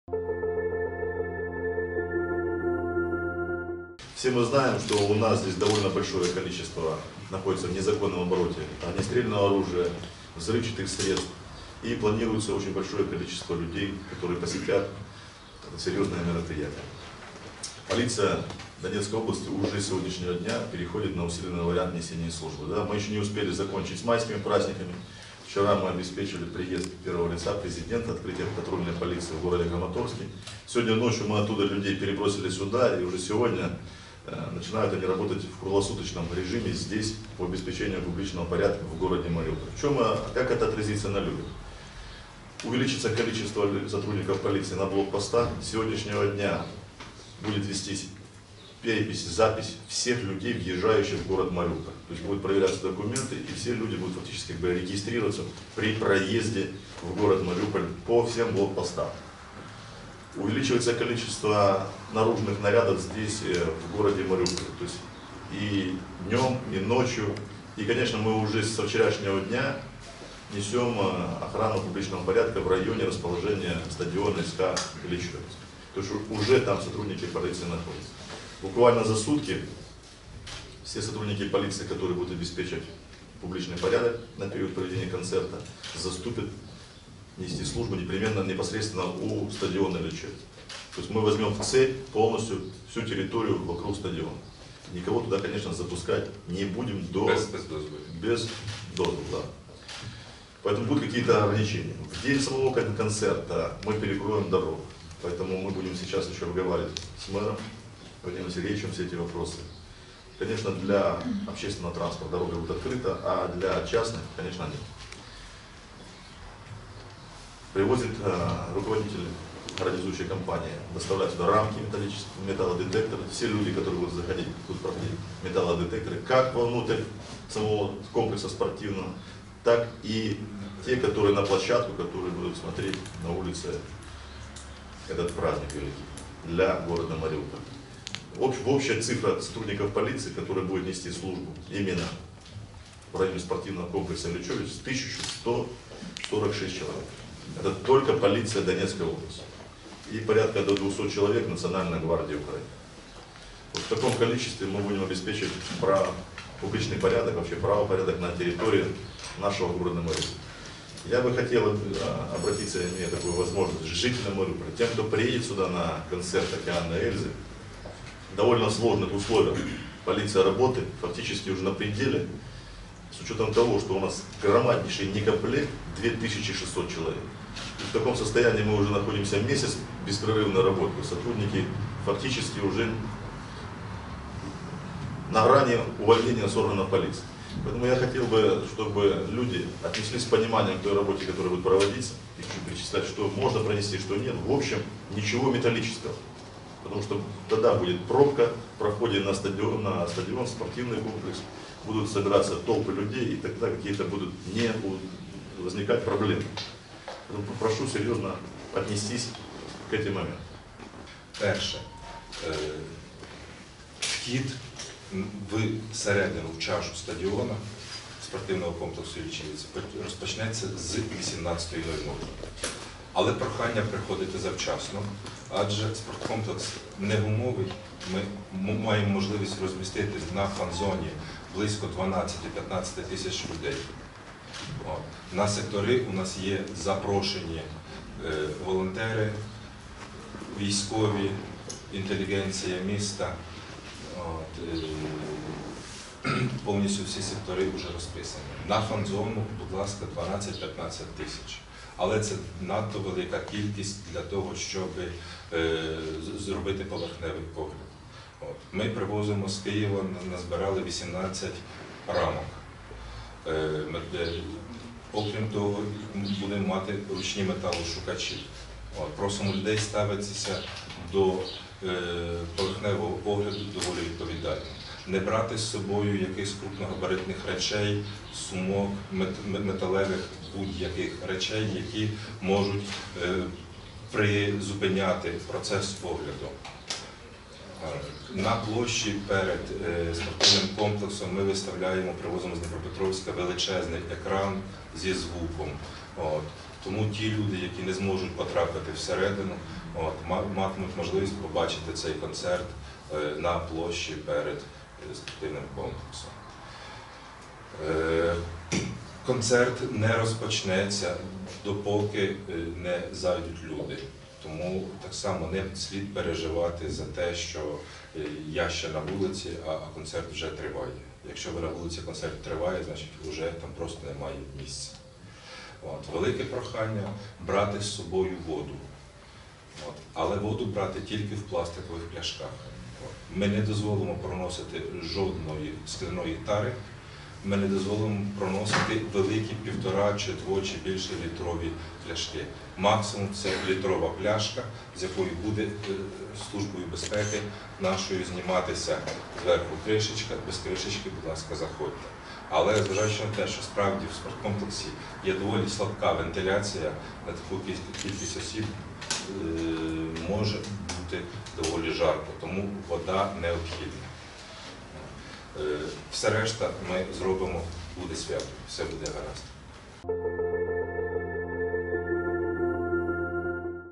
Мариупольцам рассказали о мерах безопасности на концерте «Океана Эльзы»
Во вторник, 17 мая, в Мариуполе в спорткомплексе «Ильичевец» даст бесплатный концерт группа «Океан Эльзы». В связи с ожиданием большого количества зрителей, полиция принимает усиленные меры по обеспечении безопасности на концерте.